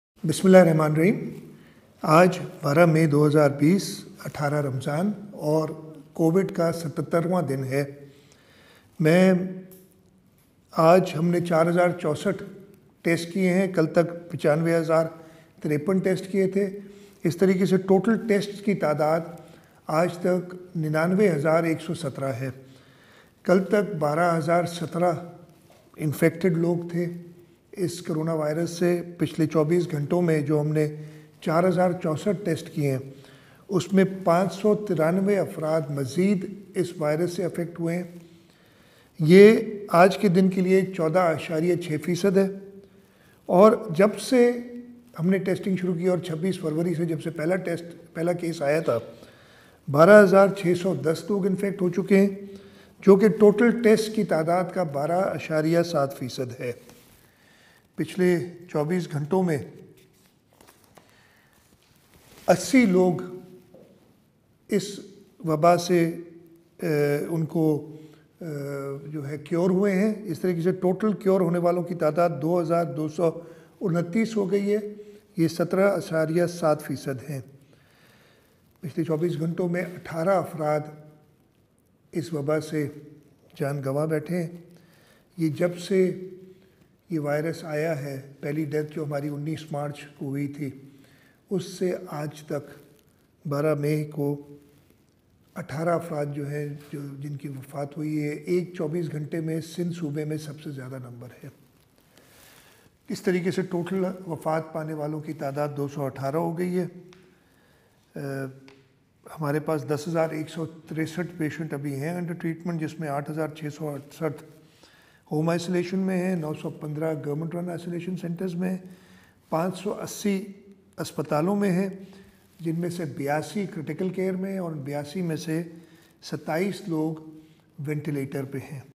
Sindh CM Syed Murad Ali Shah says COVID 19 claimed 18 lives on May 12
Sindh CM Syed Murad Ali Shah says COVID-19 claimed 18 lives on May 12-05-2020